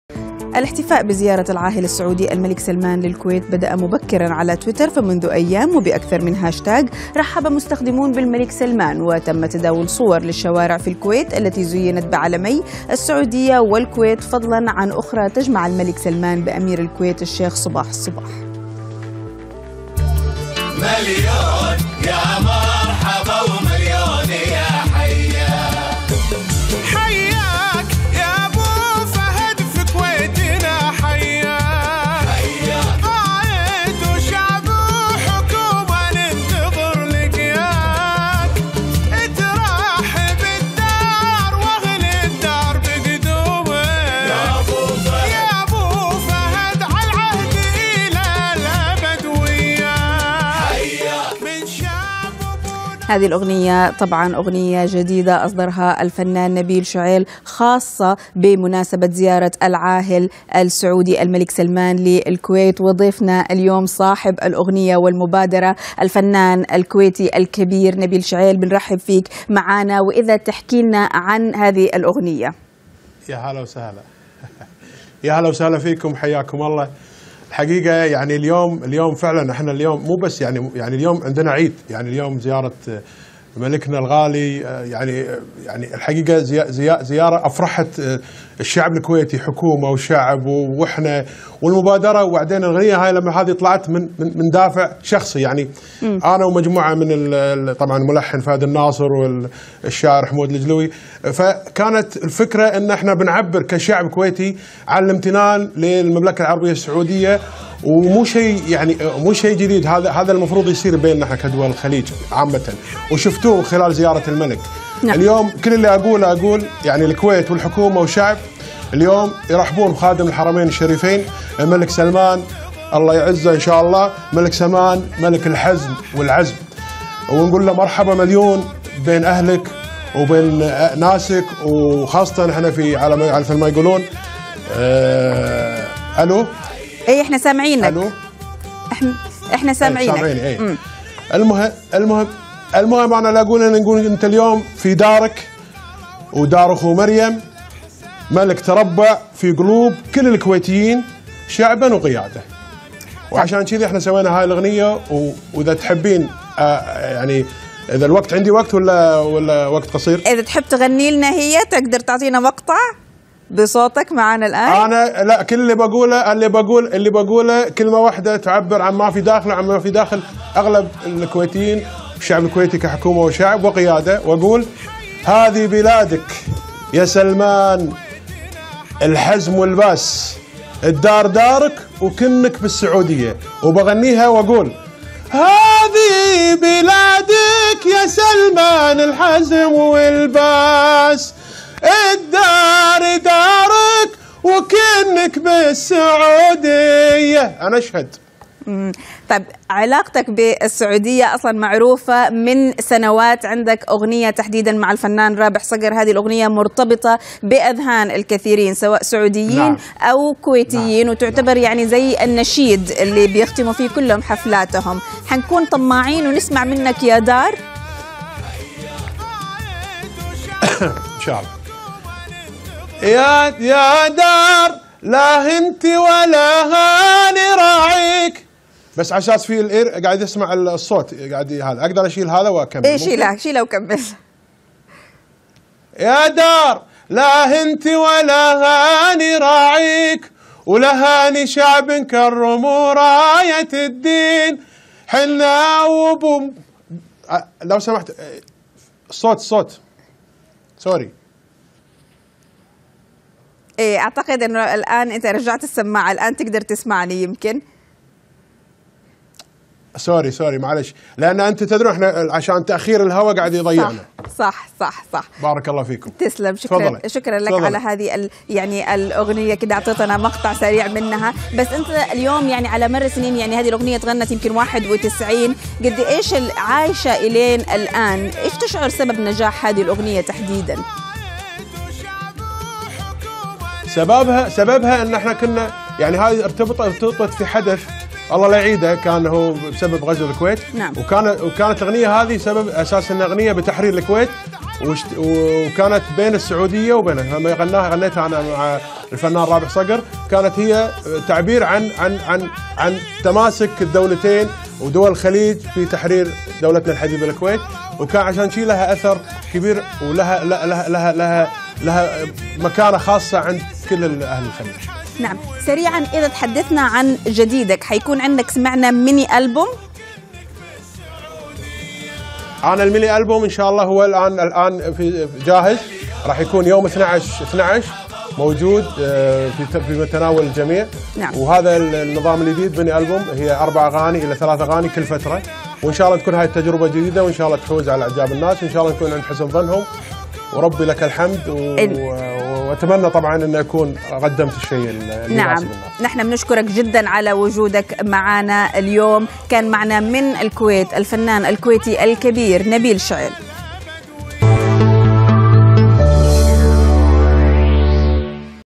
الفنان نبيل شعيل يغني في تفاعلكم ترحيبا بالملك سلمان
الاحتفاء بزيارة العاهل السعودي الملك سلمان للكويت بدأ مبكرا على تويتر، فمنذ ايام، وبأكثر من هاشتاق، رحب مستخدمون بالملك سلمان، وتم تداول صور للشوارع في الكويت التي زينت بعلمي السعودية والكويت، فضلا عن أخرى تجمع الملك سلمان بأمير الكويت الشيخ صباح الصباح..الفنان الكويتي نبيل شعيل، نشر على حساباته في مواقع التواصل، أغنية ترحيبية بالعاهل السعودي .. تعد مبادرة شخصية من الفنان الكويتي..